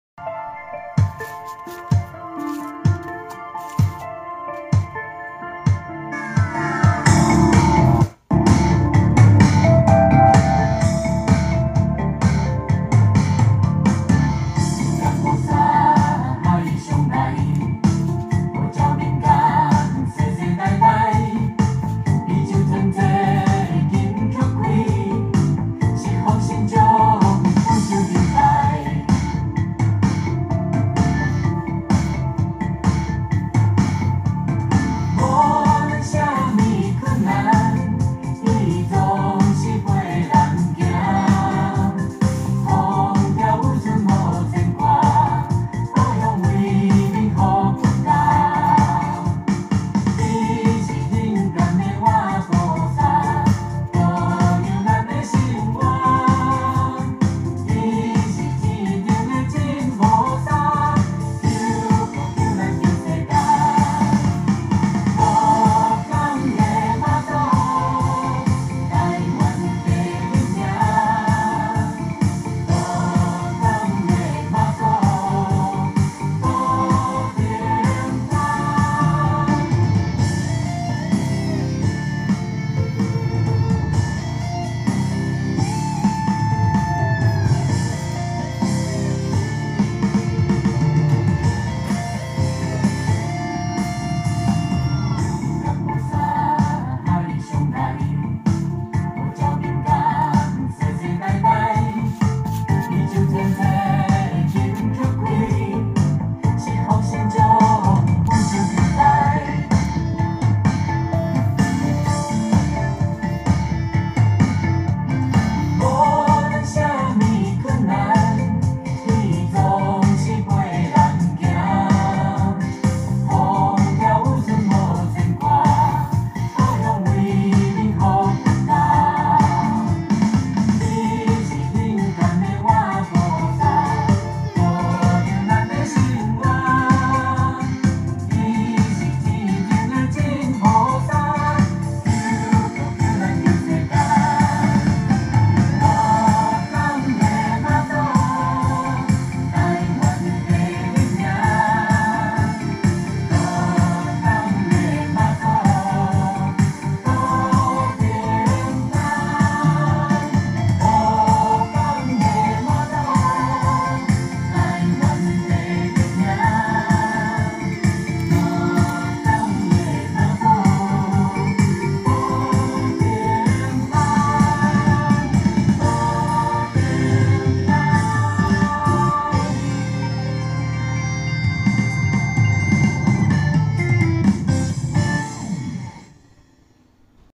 2023同安寮請媽祖
2023癸卯年
同安寮十二庄請媽祖暨第六屆媽祖文化節
🫰擇定國曆04月29、30日
💞農曆三月初十、十一日
💖埔鹽永樂村牛埔厝
                       慶安宮行臺大公舘
❤️恭請鹿港天后宮、新祖宮湄洲天上聖母
💓遶境平安。

照片/影像：咱攏係媽祖ㄟ囝吖 提供